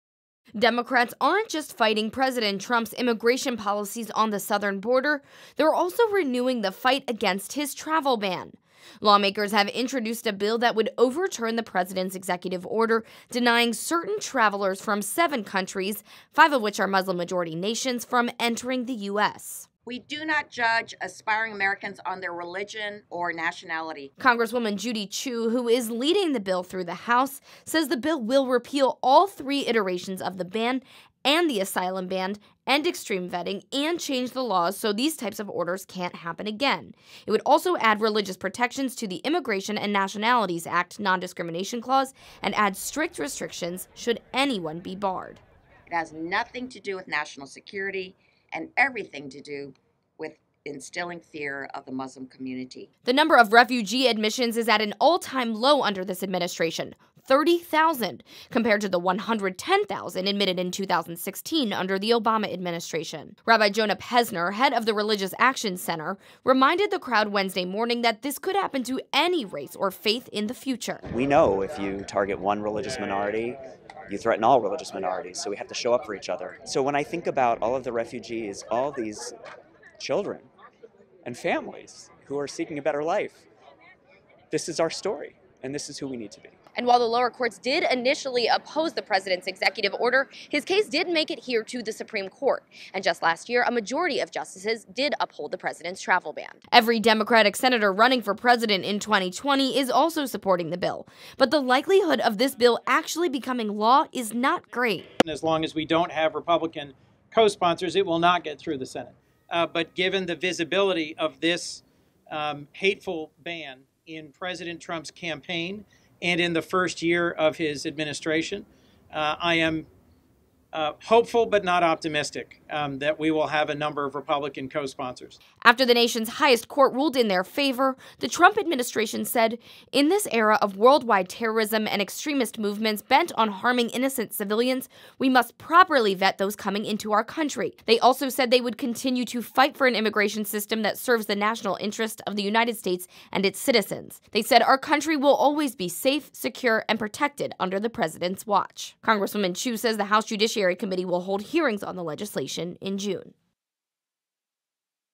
Democrats try to end Trump's travel ban
Democratic lawmakers introduced a bill Wednesday to shut down the president's executive order and prevent future ones.